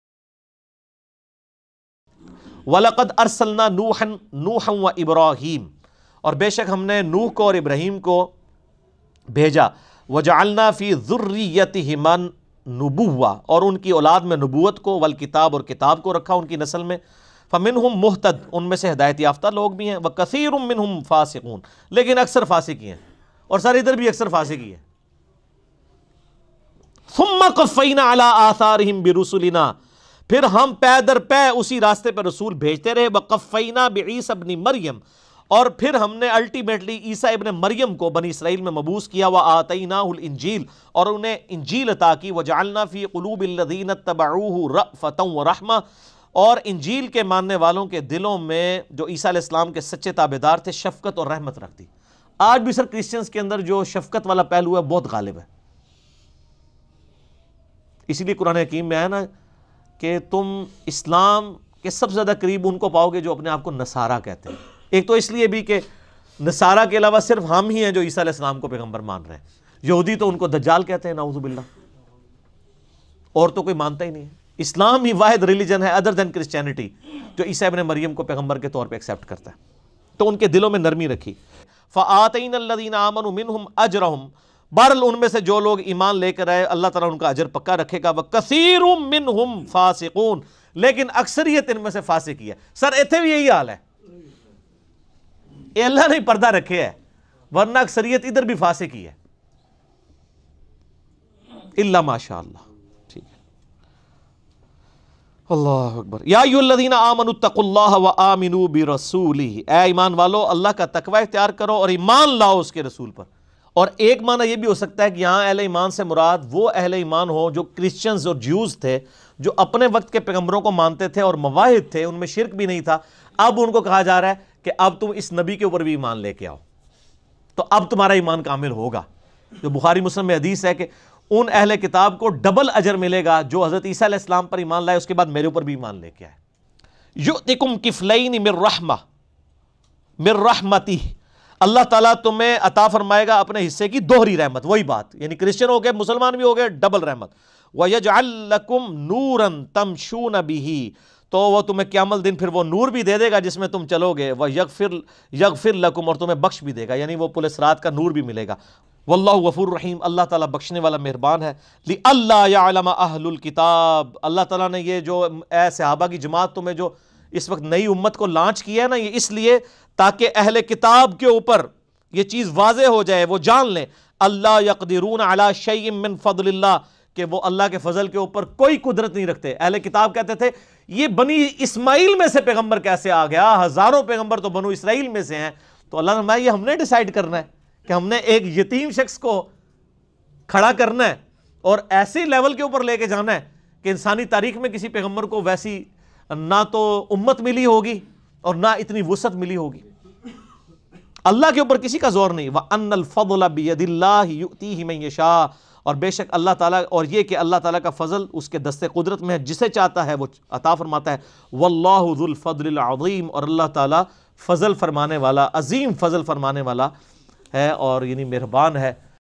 QURAN Main NASARA (Christians) Ki TAREEF (Appreciation) ??? (By Engineer Muhammad Ali Mirza Bhai)
A Critical Video Clip with JPG References From 312-Lecture of weekly QUR'AN Class (03-Feb-2019) about the Appreciation of Christians in the Holy Quran.